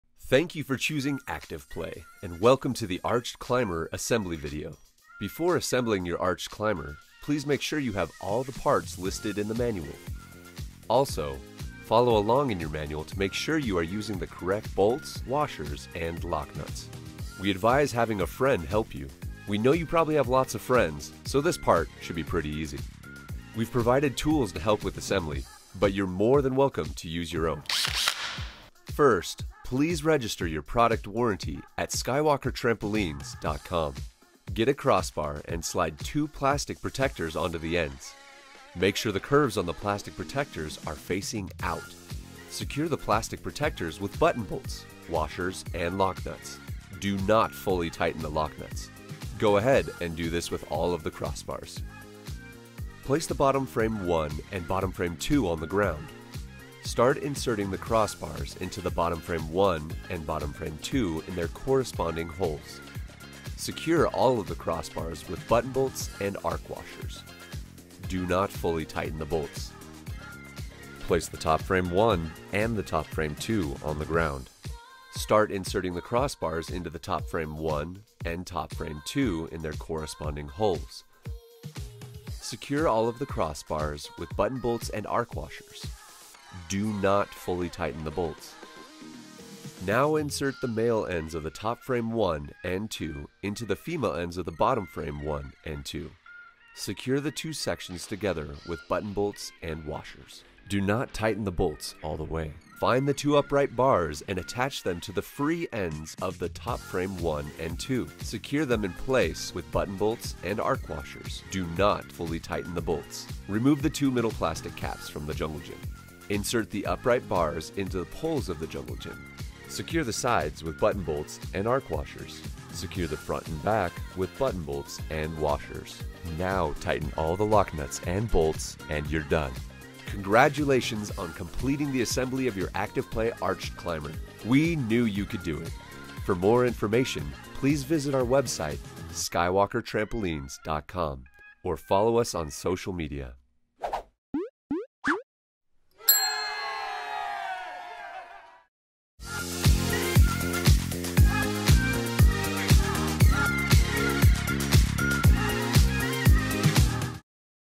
ActivPlay Arched Ladder Climber Assembly Video
Follow along with our helpful tutorials and let us give you step-by-step instructions on how to assemble your Arched Ladder Climber Accessory Module. We’re constantly improving/adding assembly videos, so check back often for updated tutorials and tips. Ready... Break! 🙌🙌🙌

⏱️NEED a SECOND LOOK at that STEP that's tripping you up?
0:00 Welcome to Assembly
0:34 Cross Bar Assembly
0:53 Bottom Frame Assembly
1:11 Top Frame Assembly
1:30 Connect Frames
1:43 Upright Bars Assembly
1:53 Connect to Jungle Gym

Didn't find the info you were looking for? No problem!
Our CUSTOMER SERVICE is top-notch!
PRODUCT: SJG205